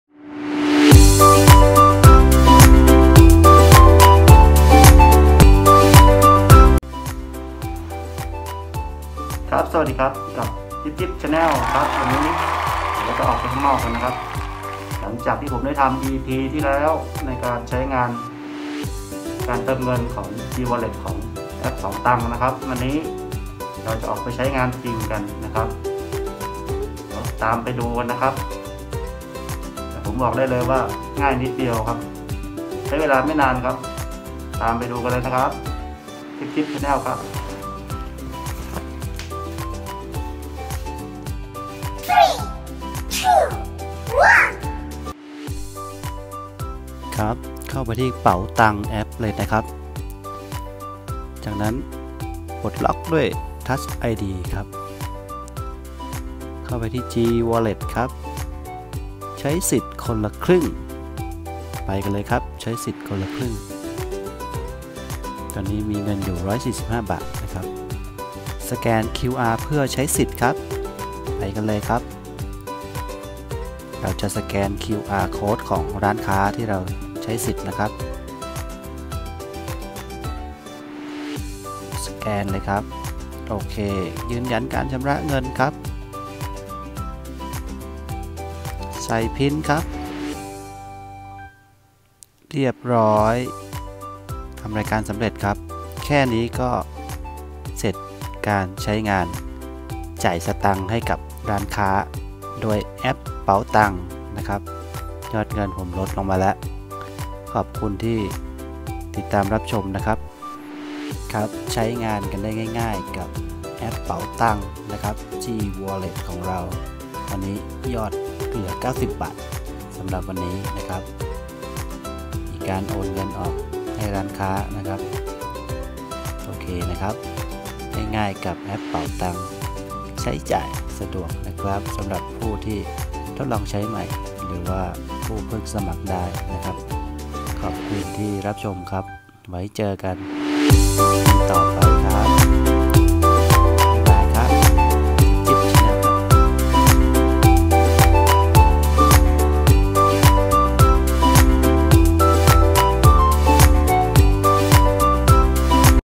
แอปเป๋าตางค์ใช้กับร้านค้า
สอนก่อนไปใช้งานจริงสำหรับคนใหม่ที่ได้สิทธิ์คนละครึ่งกับแอปเป๋าตางค์ เฟสสองและสิทธิในอนาคต ง่ายๆ ชอบกดติดตามด้วยครับ